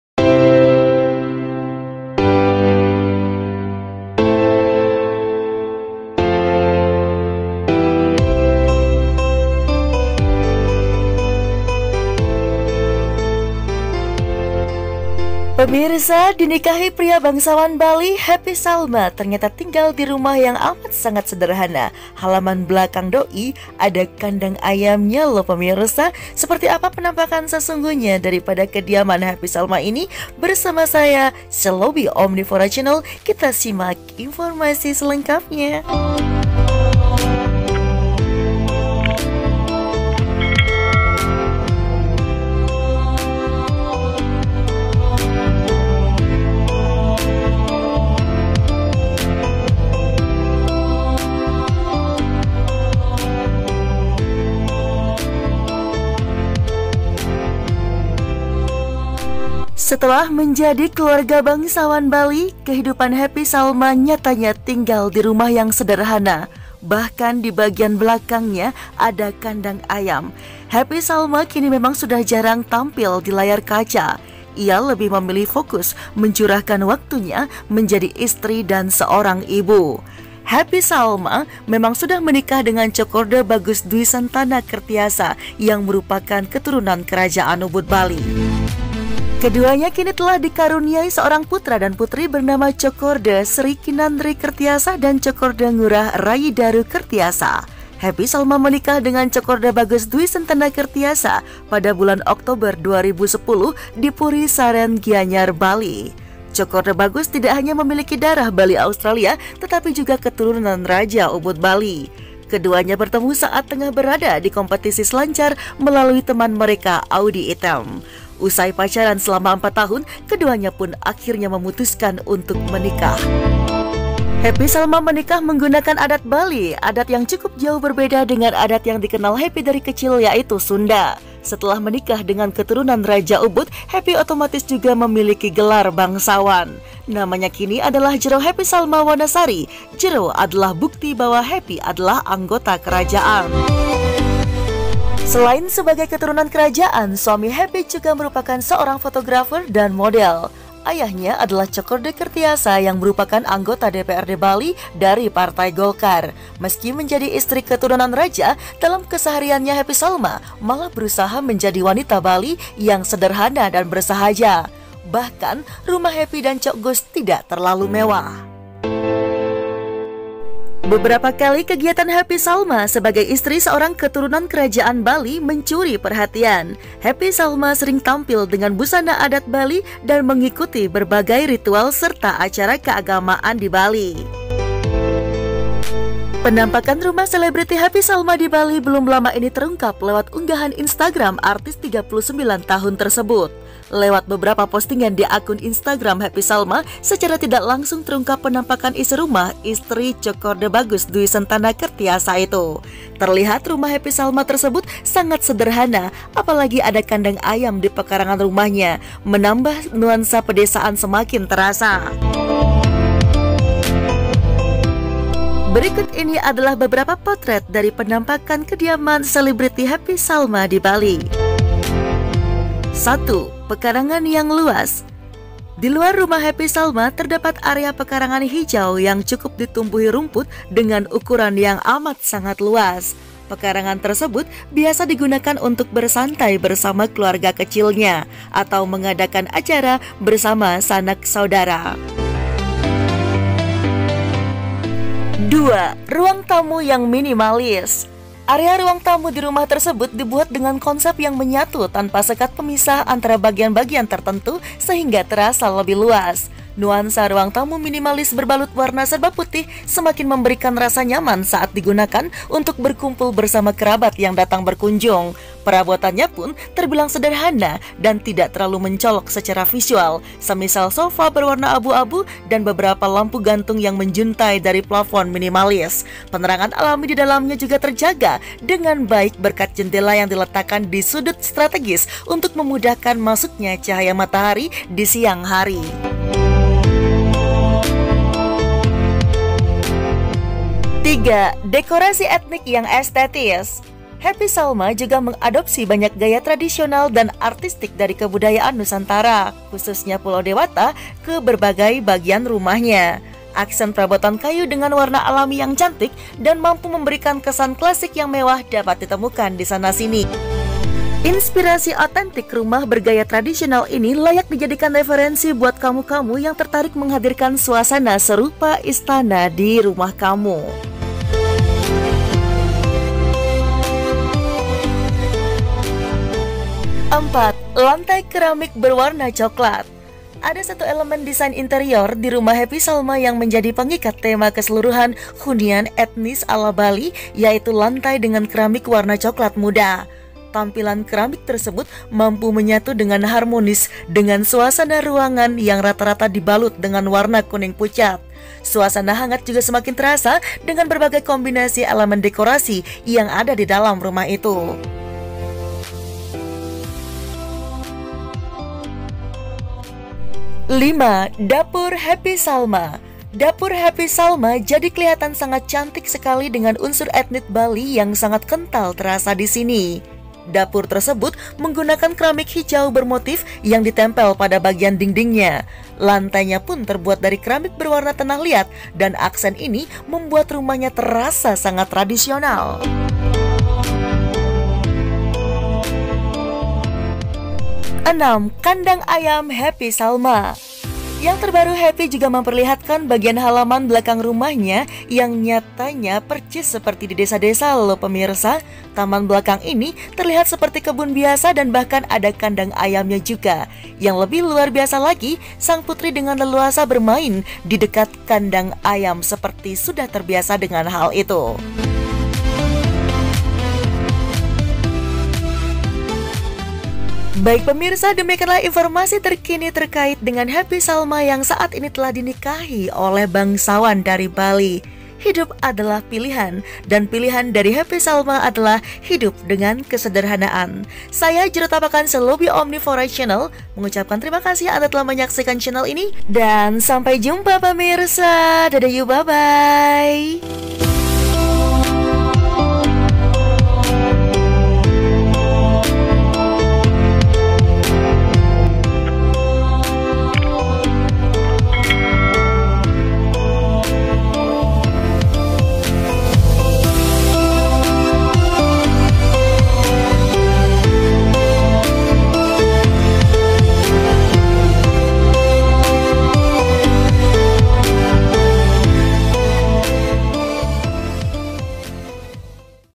KEHIDUPAN SEDERHANA HAPPY SALMA PASCA DINIKAHI BANGSAWAN DARI BALI!
Happy Salma tak lagi aktif di dunia hiburan semenjak menikah dan memiliki anak.

Seperti yang diketahui, Happy Salma dipersunting oleh seorang pria keturunan Raja Bali.

Namun, meski menikah dengan pria terpandang, rumah tinggal Happy justru nampak sederhana dan jauh dari kesan mewah.

Kehidupan Happy Salma setelah menikah dengan keturunan kerajaan Bali, Tjokorda Bagus Dwi Santana Kerthyasa sering jadi sorotan.